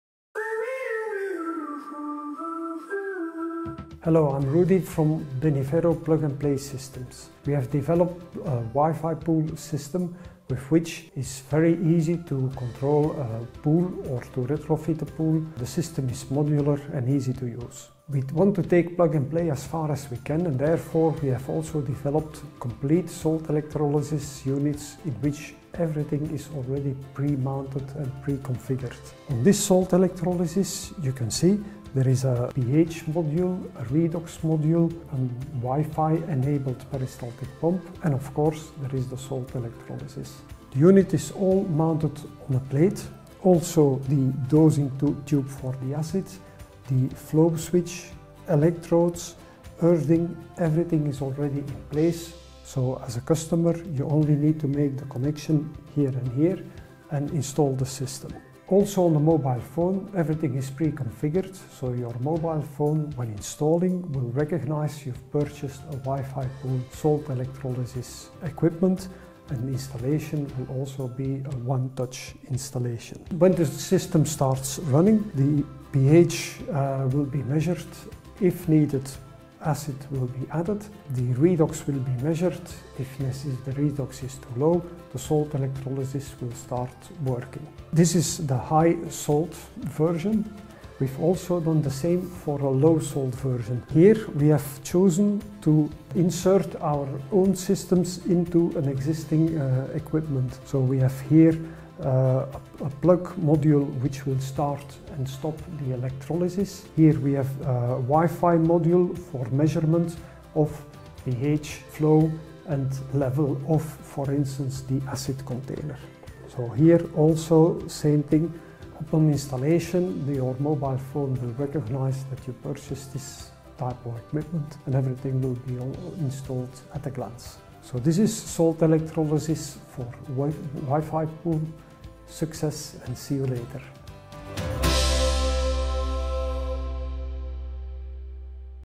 Beniferro Wifipool salt electrolysis ENG
Learn how you can easily and chaeply control your swimming pool salt electrolysis using the Wifipool app and hardware from Beniferro.